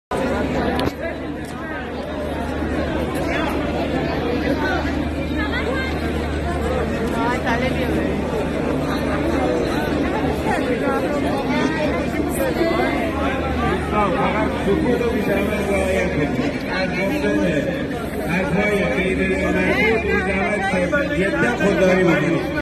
رشت
رشت - تجمع اعتراضی نیروهای شرکتی مخابرات گیلان ـ
نزدیک به ۲۰۰ نفر از نیروهای شرکتی مخابرات گیلان صبح امروز با تجمع در  ساختمان مرکزی مخابرات استان، خواستار رسیدگی به مطالبات حقوقی خود شدند.